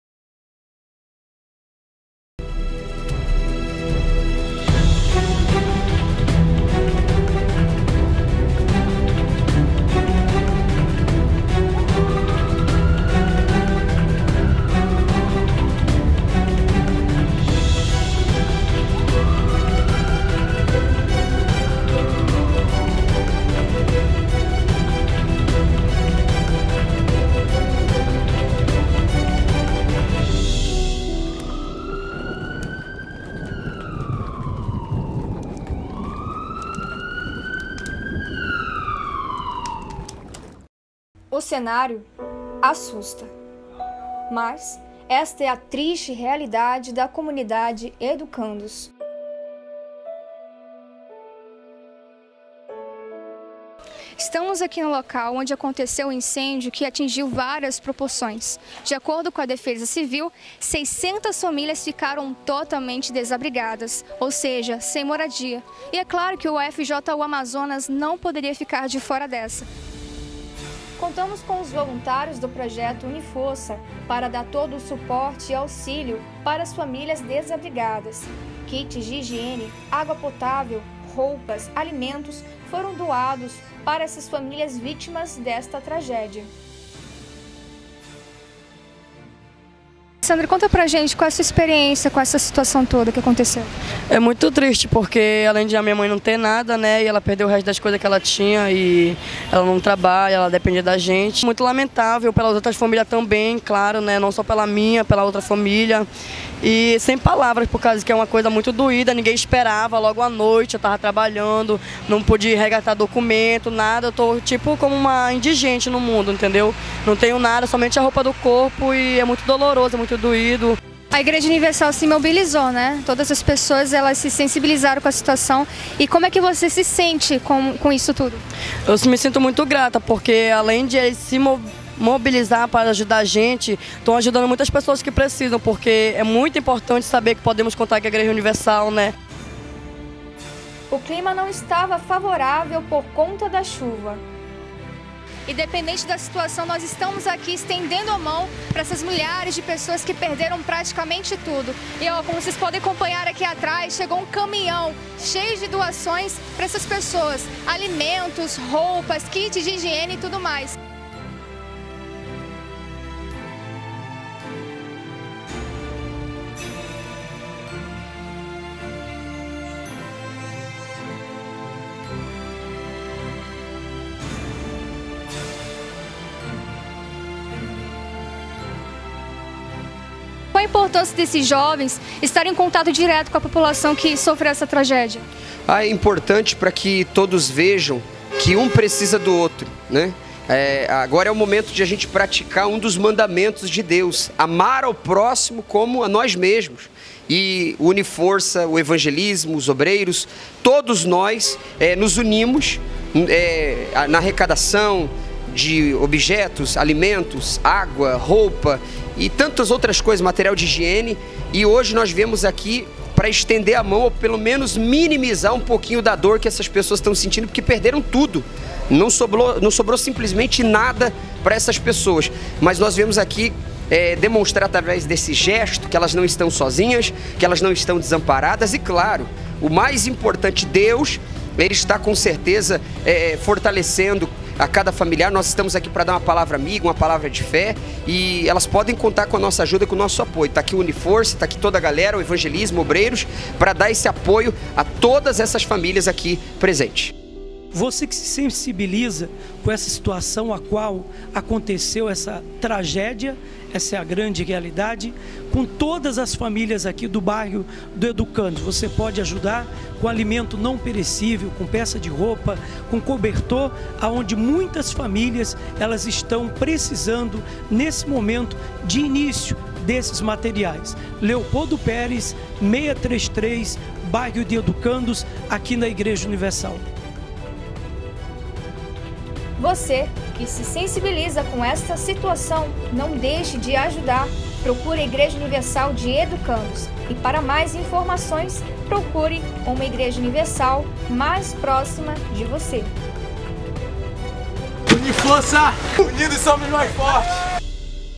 SOS Educandos - Amazonas
Acompanhe a ação social que a FJU de Amazonas proporcionou à comunidade em Campos.